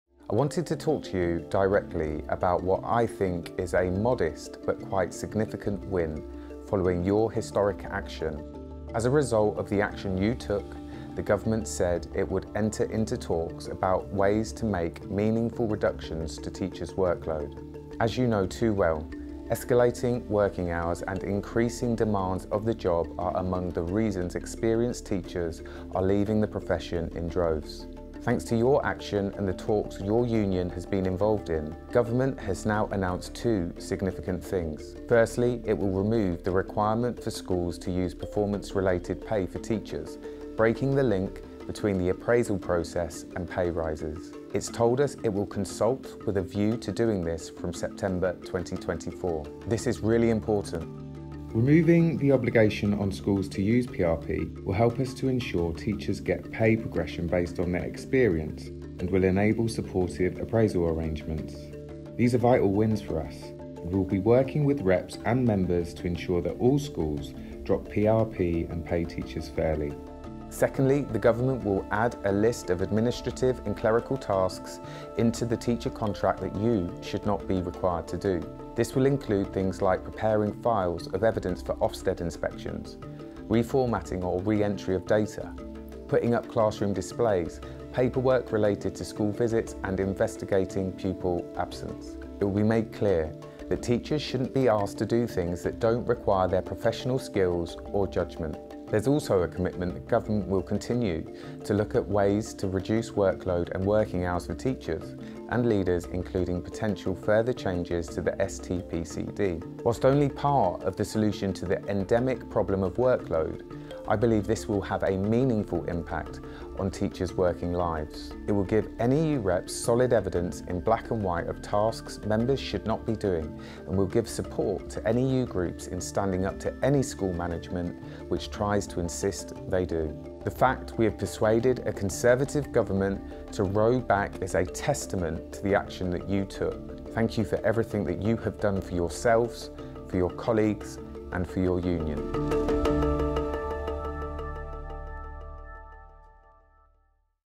Daniel Kebede, NEU gen sec's message to members on workload and PRP.
The Government announced it will remove performance related pay (PRP) for teachers and take steps to reduce workload and working hours.